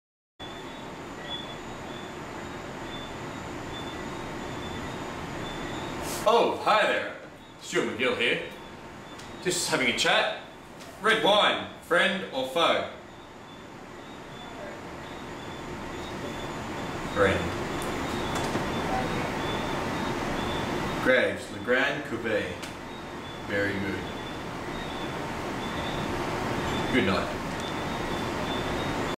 doug bollinger previews stuart macgill wine show.wmv
From next month (thank-you Google plus!) I'll be back on the wine trail... its been a while though and I'm a bit nervous about getting back in front of the camera. I called in my good mate Doug Bollinger to remind me how its done...